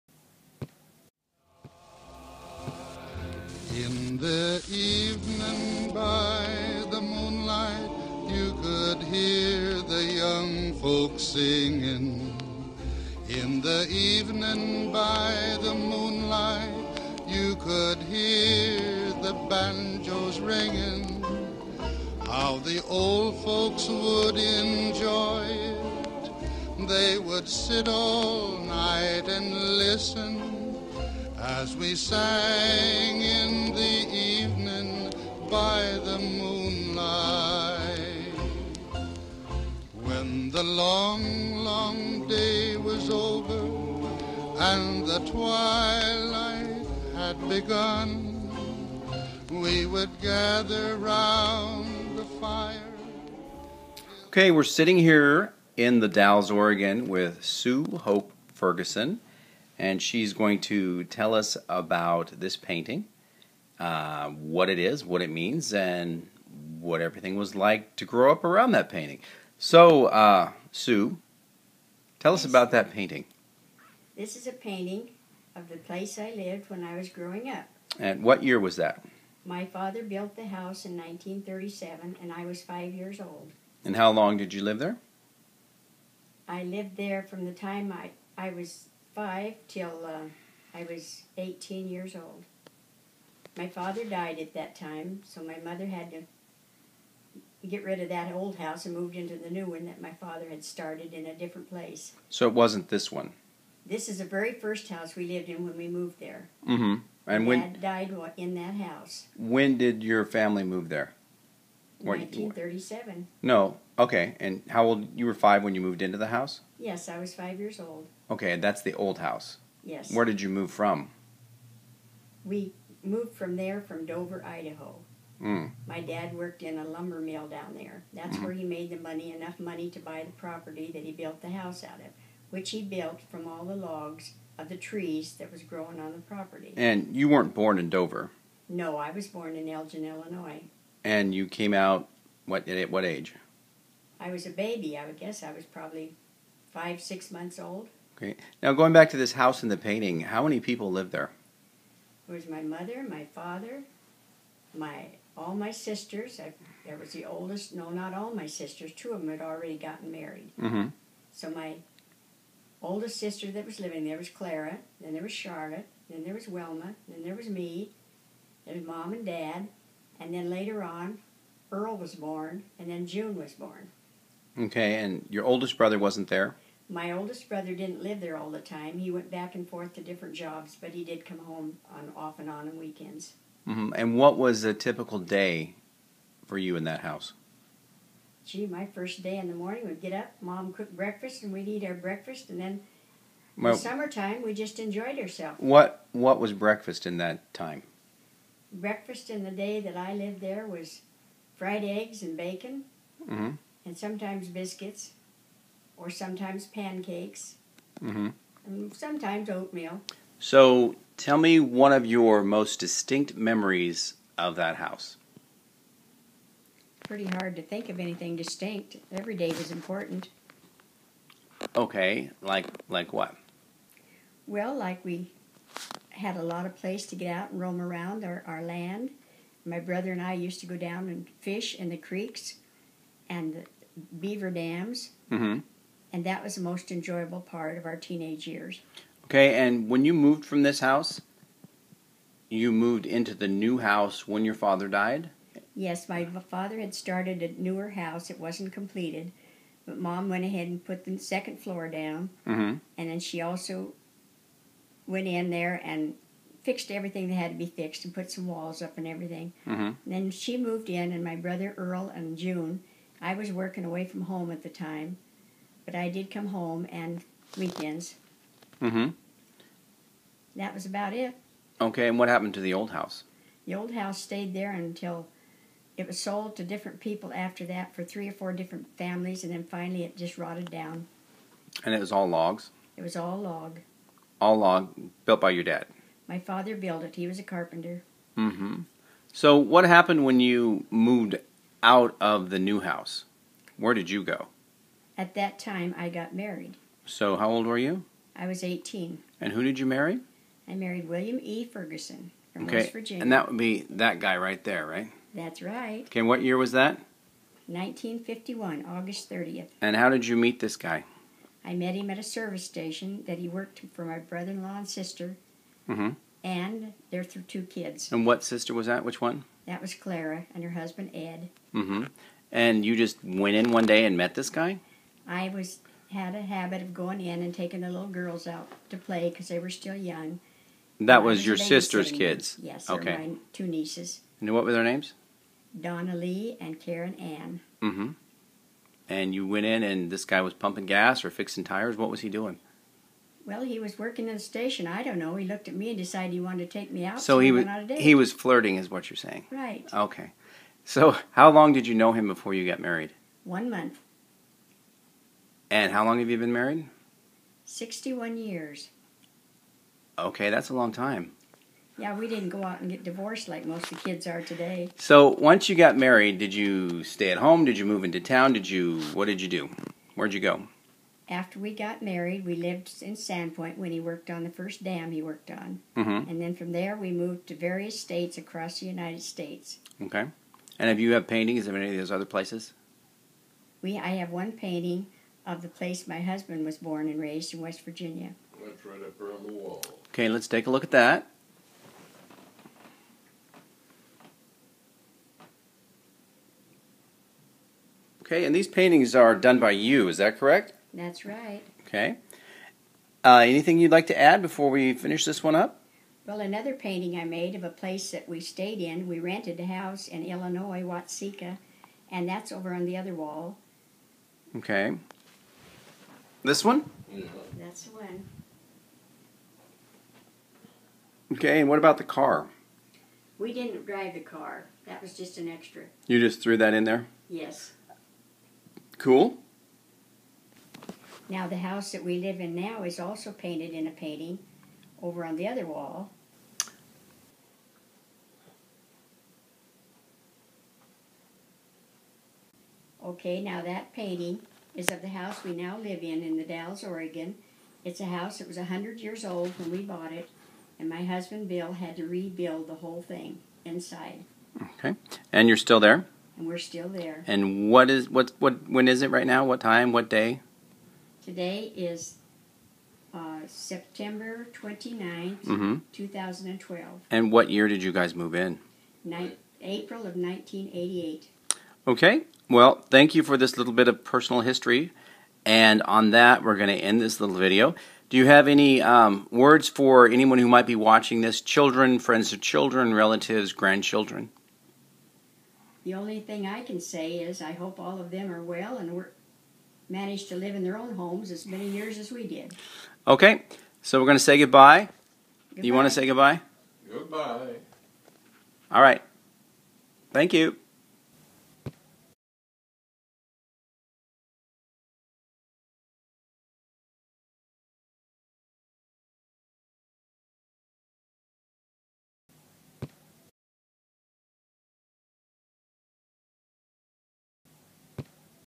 Mothers Homestead
Sue Ferguson talking about her childhood home in the country setting surrounding Sandpoint, Idaho.